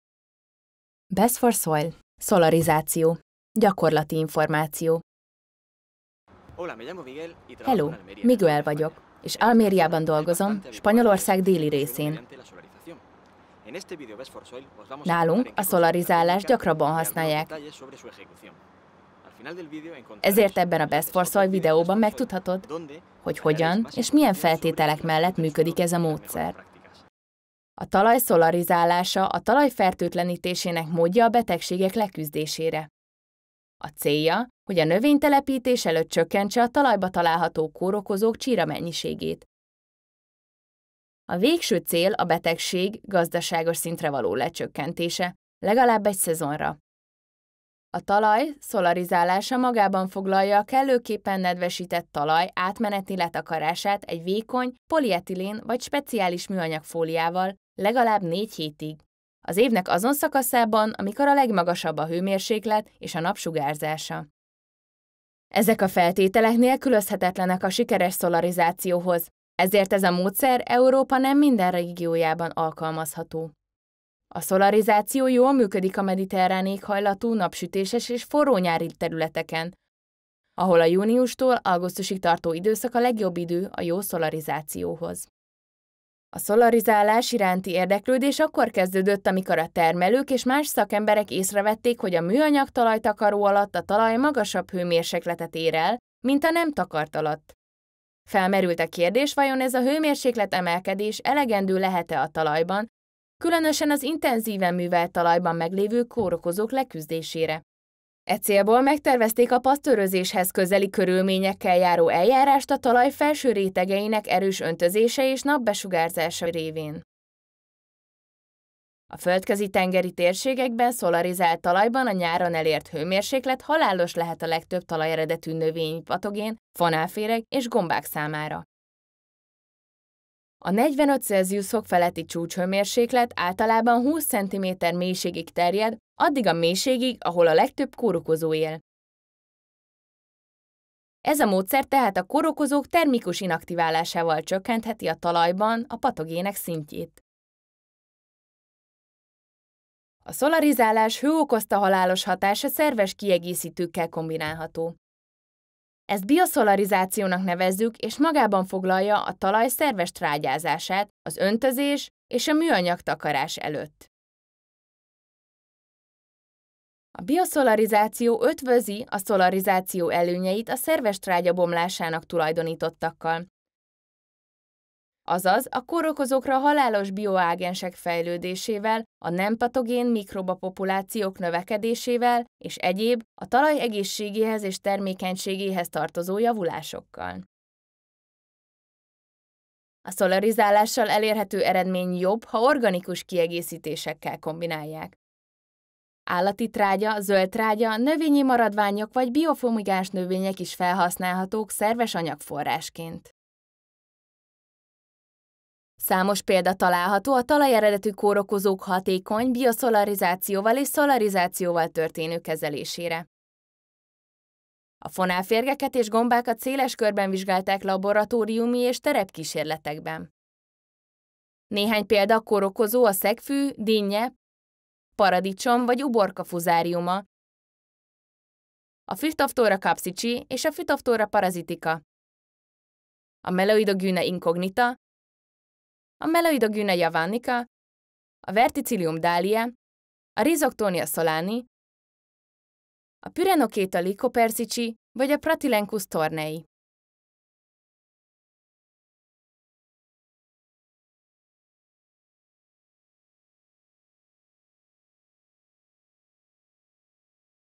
(Bio)szolarizáció - Gyakorlati tudnivalók
A szolárizáció önmagában vagy szervesanyag adalékokkal kombinálva, az úgynevezett bioszolarizáció, egy Dél-Európában gyakran alkalmazott talajfertőtlenítési eljárás. A hatékonyságáról és alkalmazásáról gyakorlati információkat talál ebben a videóban.